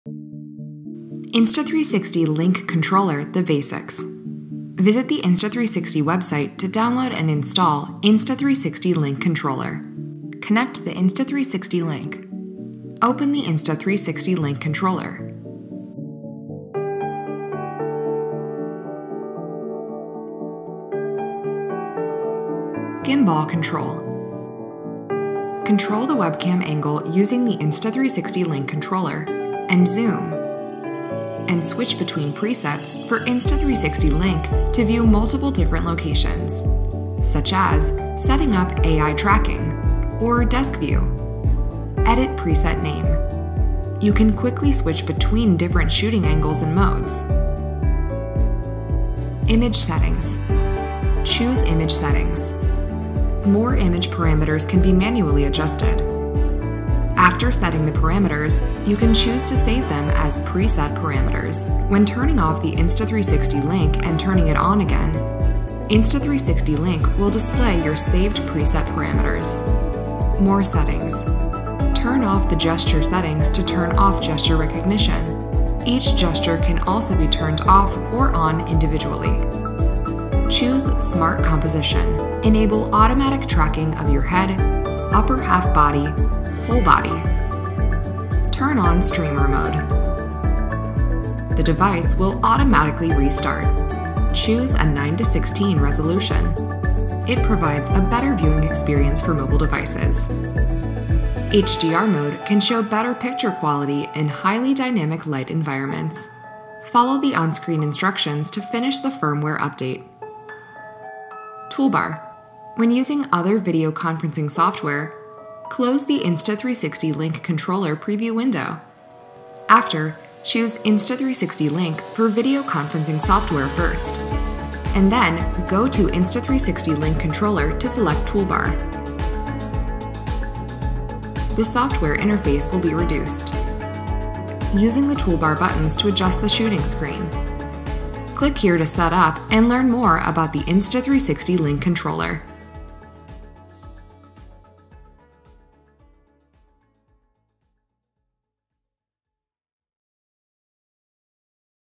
How to Update Insta360 Link Controller | Insta360 Link Tutorial
Insta360 Link, the AI-powered 4K webcam is packed full of unique features to help you work smarter! This tutorial will show how to update the firmware to utilize Link’s full potential. From downloading the firmware using the Link Controller to manually downloading from the official Insta360 website, you’ll be set to revolutionize the way you work!

00:04 Download and Install Insta360 Link Controller
00:23 Gimbal Control
00:49 Image Settings
01:10 More Settings
01:52 Toolbar